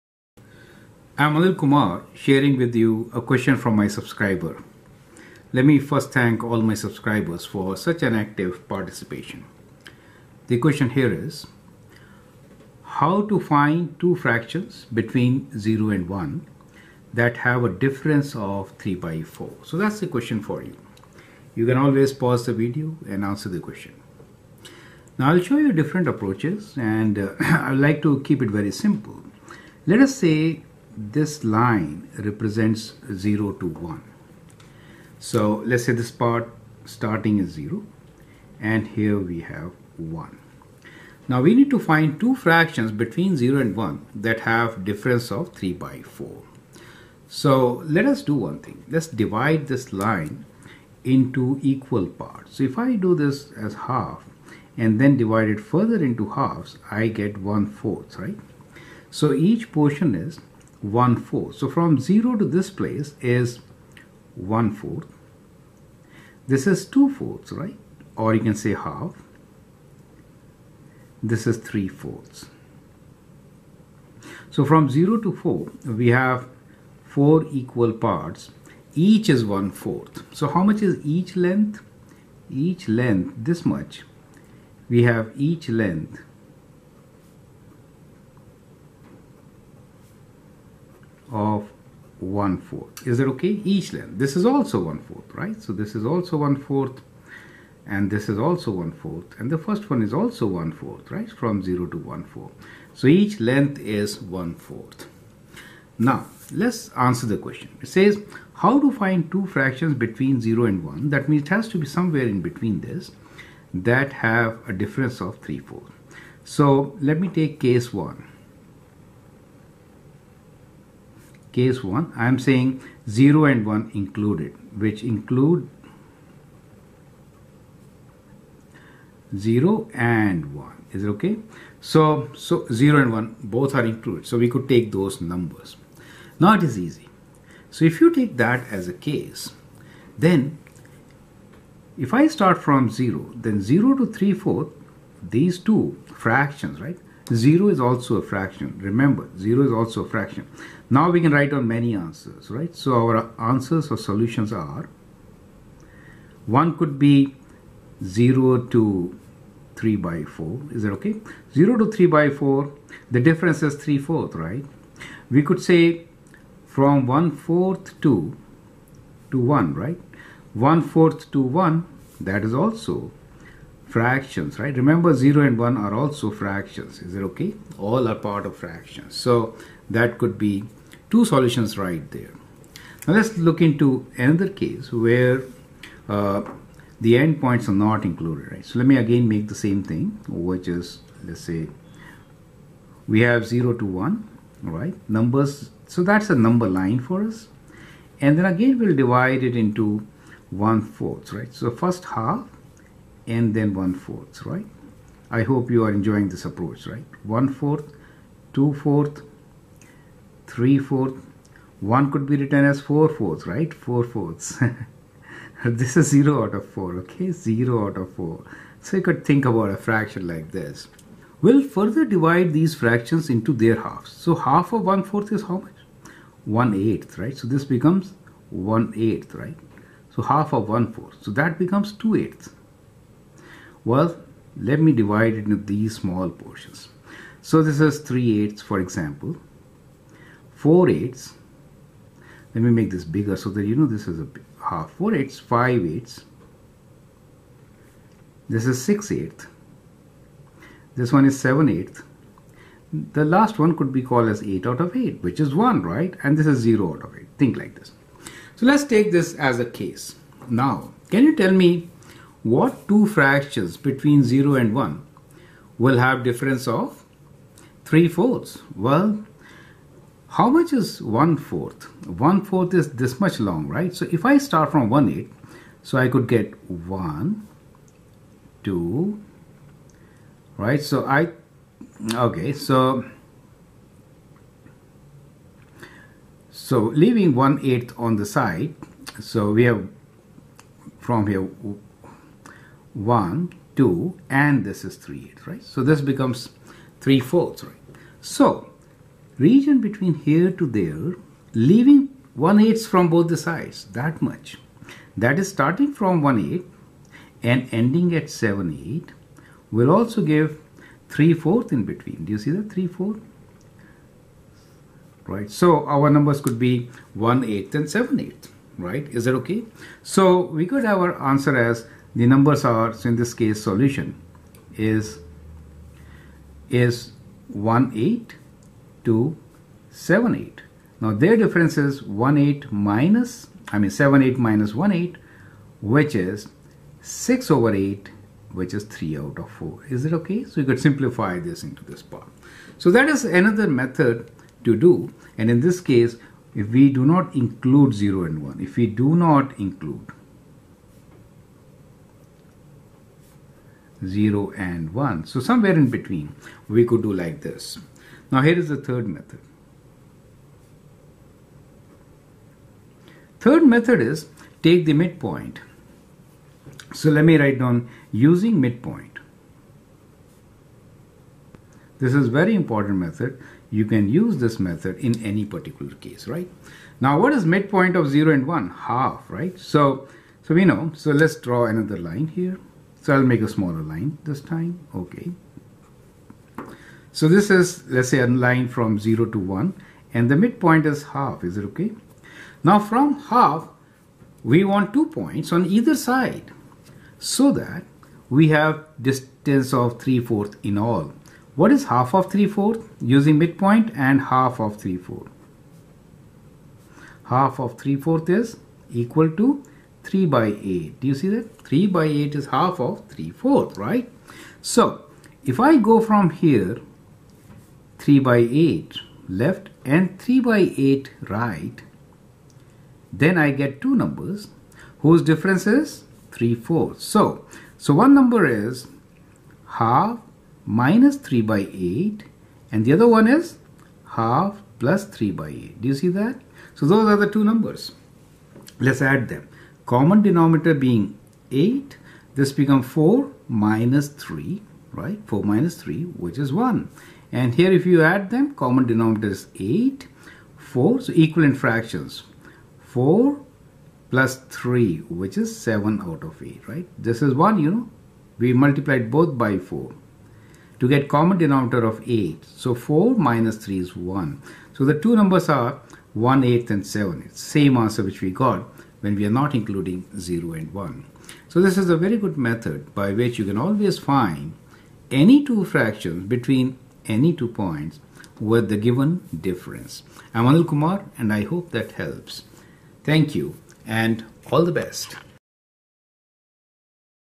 Find fractions between zero to one with differnce of three fourths
Improper fraction: numerator is greater than denominator. They are greater than equal to 1.
Fractions are of the form p/q where q is not zero. If p is greater than q then the fraction is called improper fraction. Value of improper fraction is more than 1. We can divide the numerator by the denominator and then write it as a mixed number. Visual illustrations in this video help to understand the concept clearly in a very simple language. Video can help teachers and instructor to explain the concept. Thanks.
Fraction represents part of the whole
Proper fractions lies between zero and one.
between any two fractions we have infinite fractions